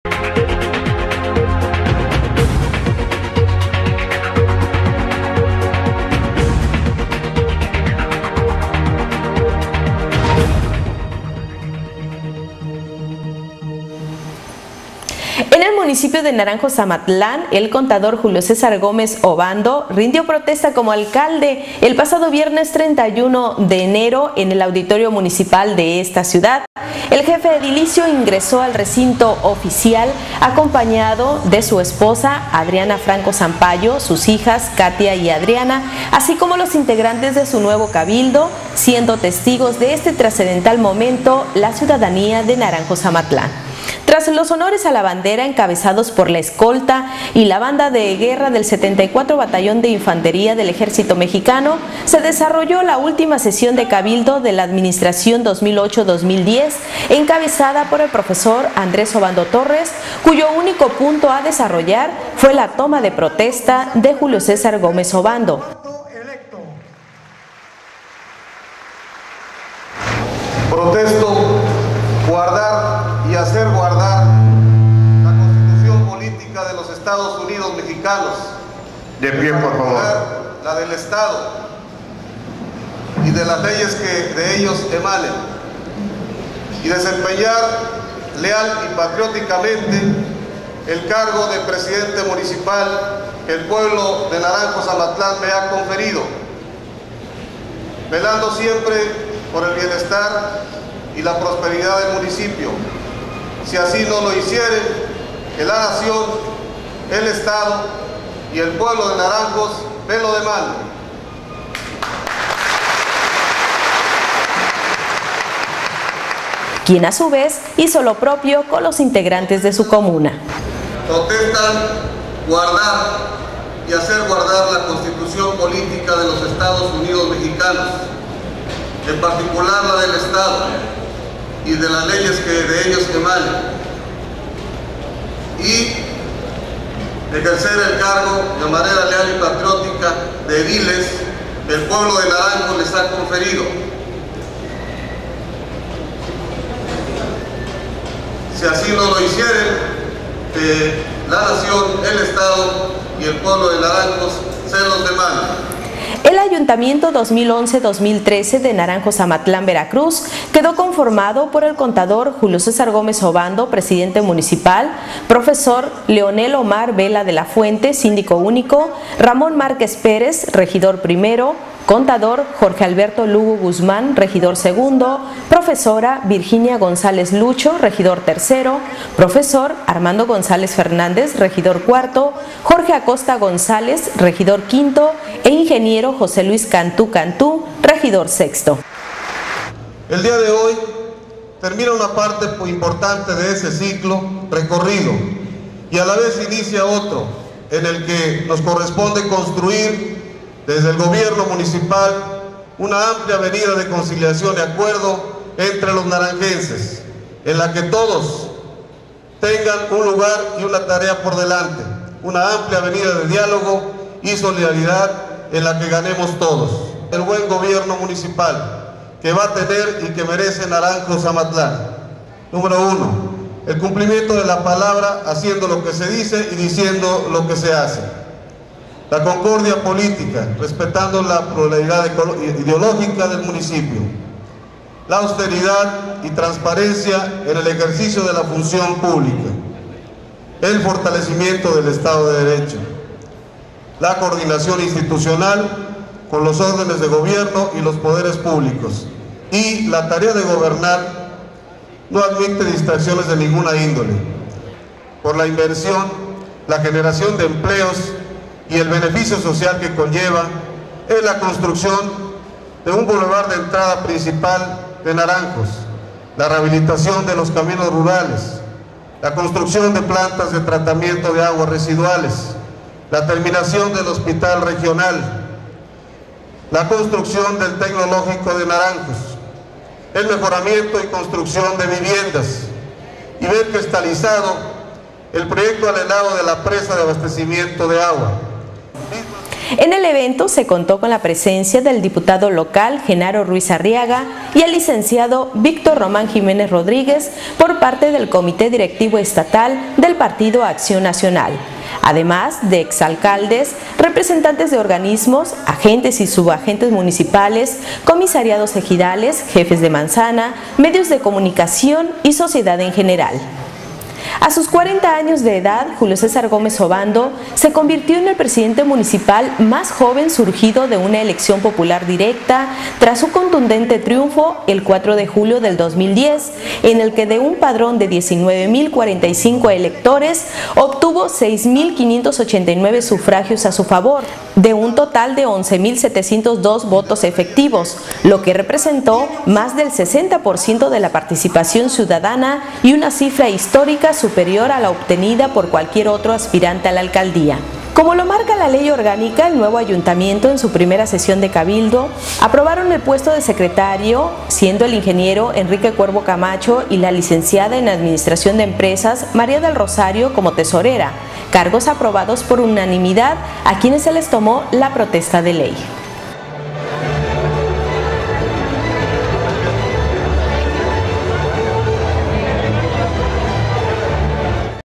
Julio Gomez Obando, rinde protesta como presidente de Naranjos-Amatlán, Ver..mpg
Julio César Gómez Obando rindió protesta como alcalde del municipio de  Naranjos Amatlán, Veracruz el 31 de diciembre, comprometiéndose a hacer del suyo un gobierno plural e inclyente, mirando en todo momento el desarrollo de todos lo sectores